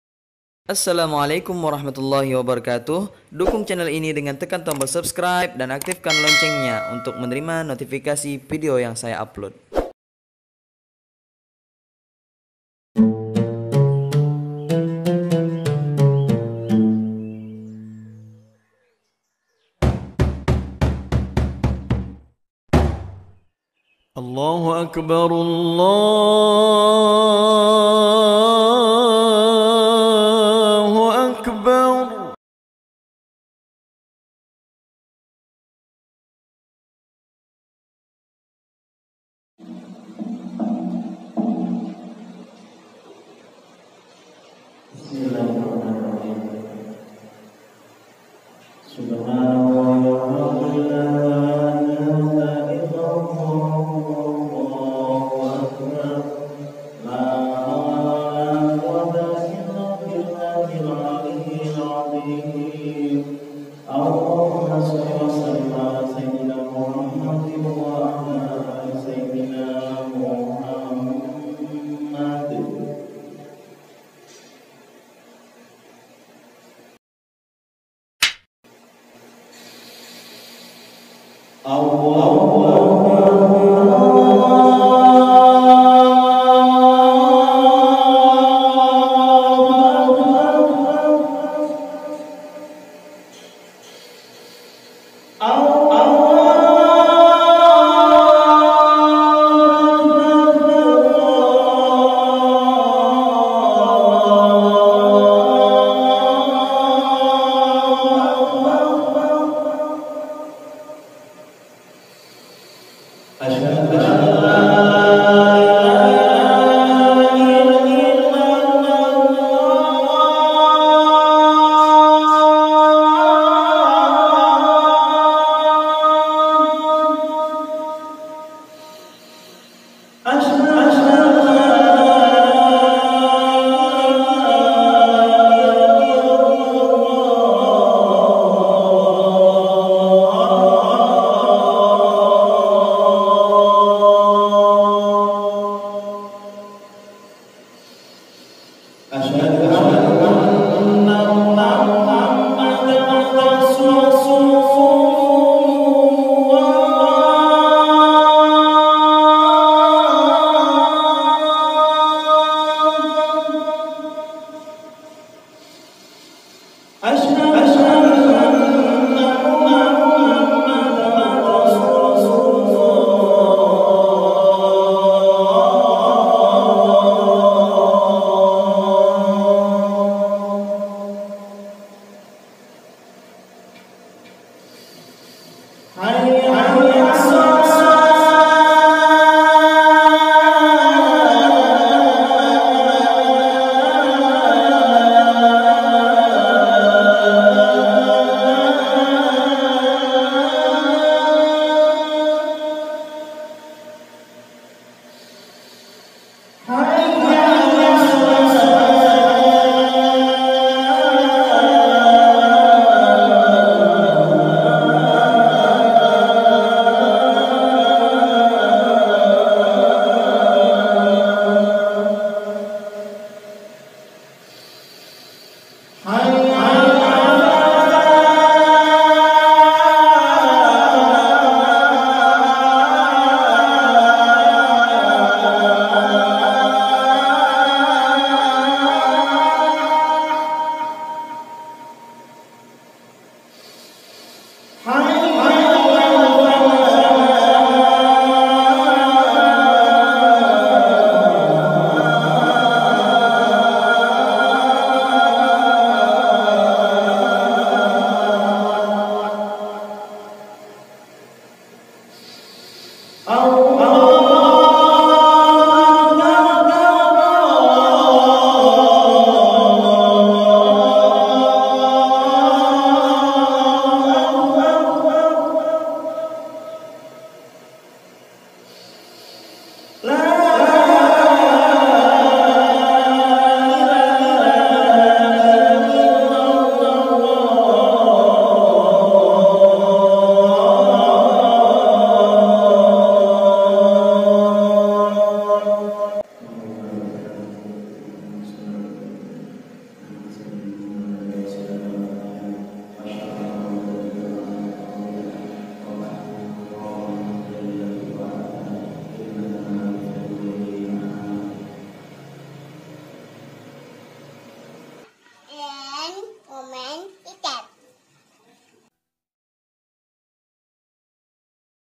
Viral !!! Adzan Jiharkah Merdu Termirip Syekh Abdul Karim Al Makki By Ustad Kamal
adzan viral yang paling sukai anak anak khususnya di serial kartun upin ipin